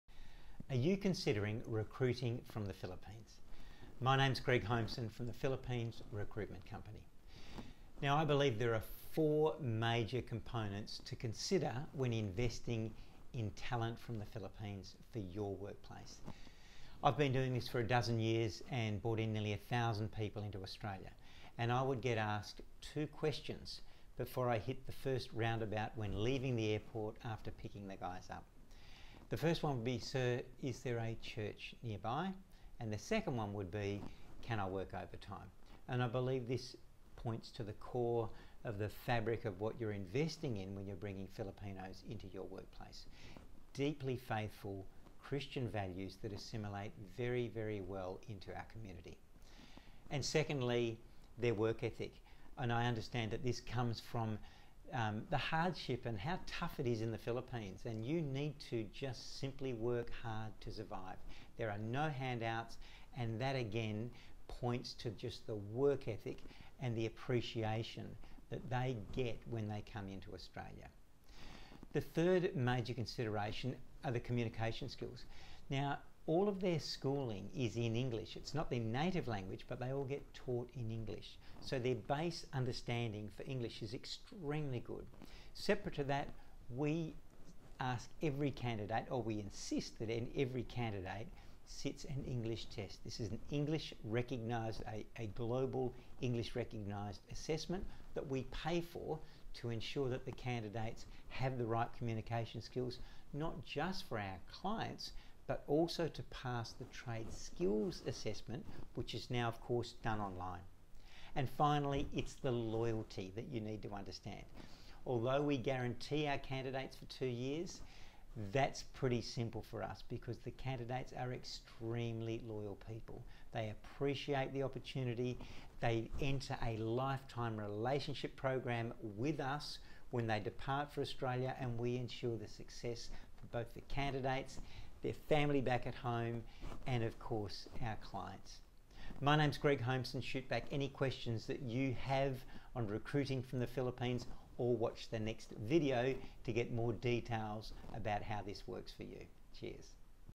What you get when recruiting from the Philippines
We help business owners reduce recruitment, advertising, and training costs by hiring amazing talent from the Philippines.

We have our own team based in Manila who worked directly with our clients.

Our strengths lie in our experience. Our ability to recruit excellent candidates is unmatched and we bring value beyond the basic search function and recruitment of candidates.

Our team understands your need for skills and productivity. That’s why our recruitment process is thorough. We spend the time getting to know your recruitment needs and then take candidates through the following process:

CV assessment
Initial discussion/pre-screening
Interview and agency introduction
First technical assessment
Document preparation for Trades Recognition Australia (TRA) – if required
Second technical assessment and profiling
Final interview and placement

TPRC offers a commitment that your recruit will be with you for a minimum of 2 years